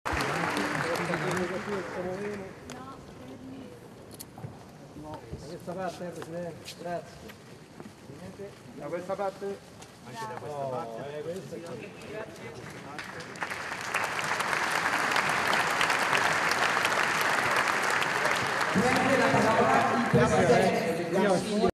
La stretta di mano tra Meloni e il Presidente di Confindustria Orsini all'Assemblea del gruppo
(Agenzia Vista) A seguito del suo intervento di apertura dell'assemblea di Confindustria il Presidente Orsini ha salutato la premier Meloni seduta in prima fila. I due si sono stretti la mano salutati da un grande applauso.